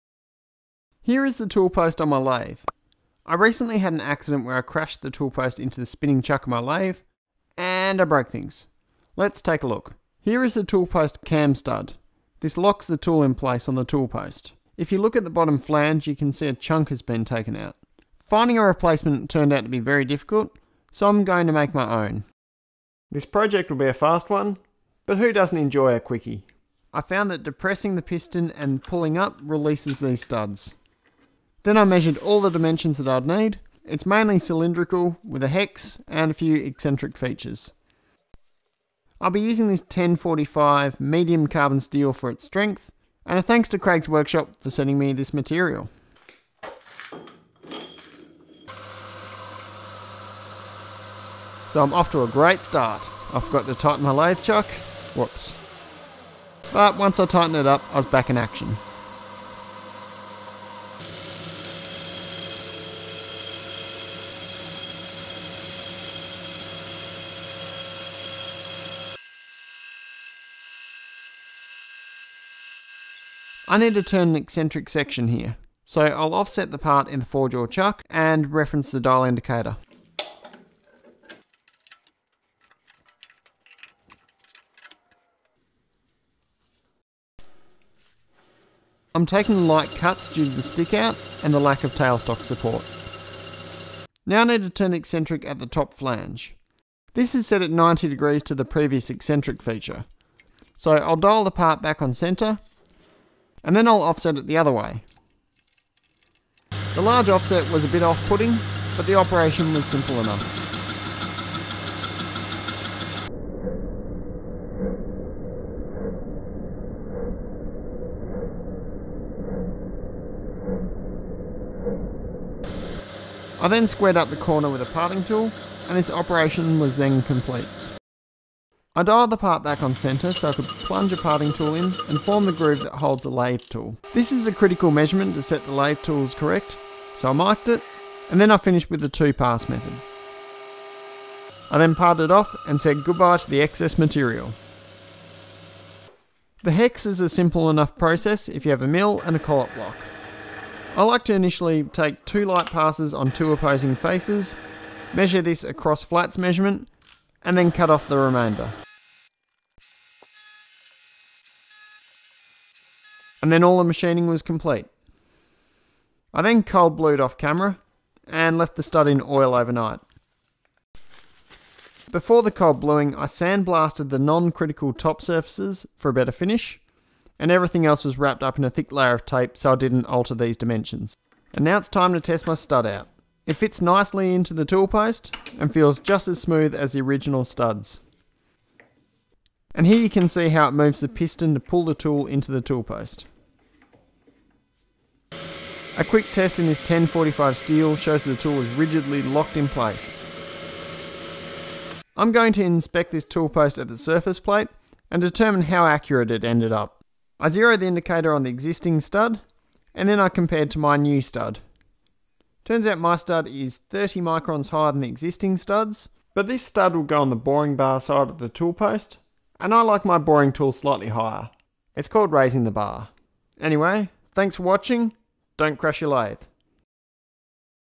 This will fix my lathe
So... I accidentally crashed my lathe and broke a quick change tool post cam lock stud. 😬
You can't really buy these as a replacement part.

How did I crash it?!
I was threading a part, then had a lunch break... I came back, put the lathe in high gear for the next operation and didn't disengage the threading lever...
My tool slammed into the chuck and broke the stud.
I thought it was a good chance to play with an interesting eccentric part
I've not really done any eccentric turning so this was fun.